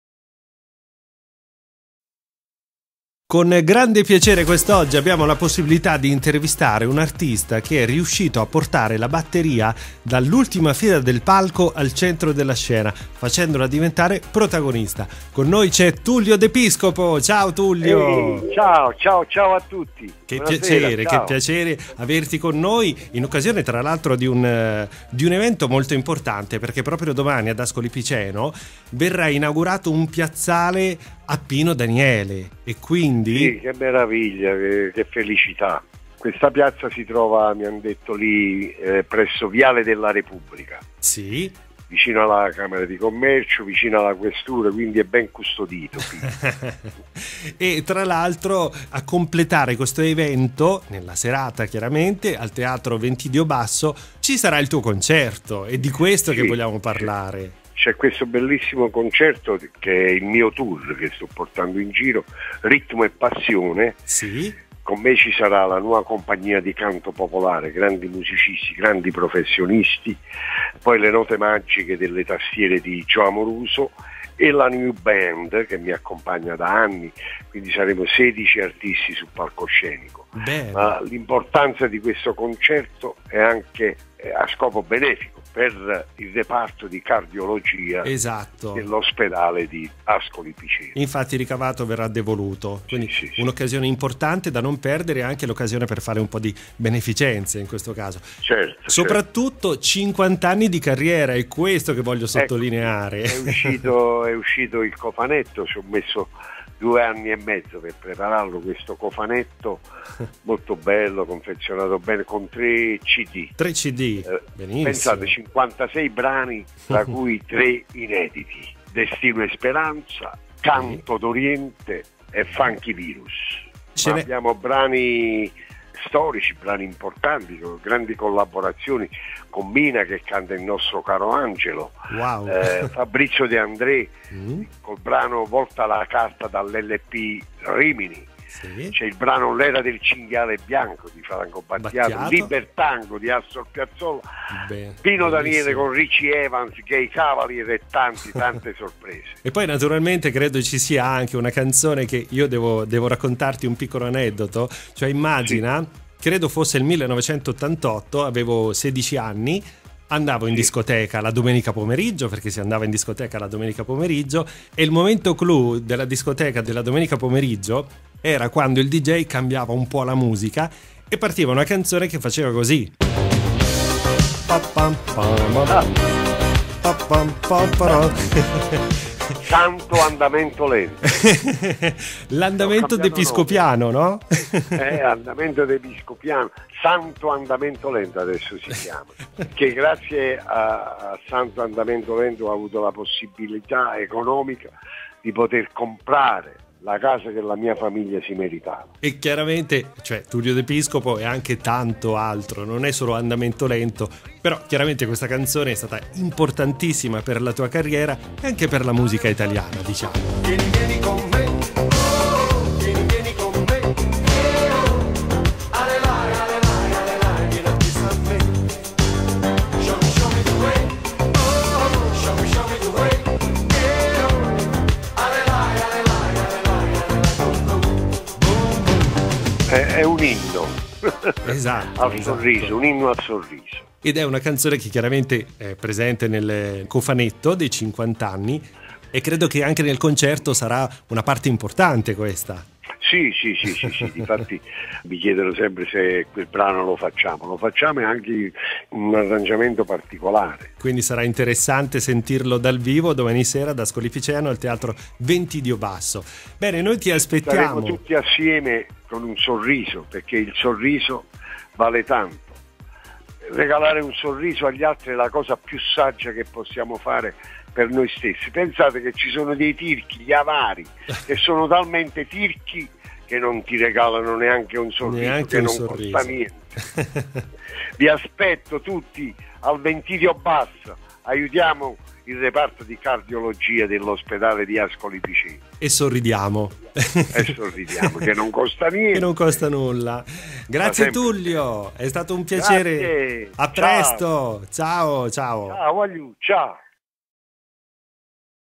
Intervista: Tullio De Piscopo
Tullio De Piscopo ospite su Radio Azzurra in occasione del suo concerto ad Ascoli Piceno.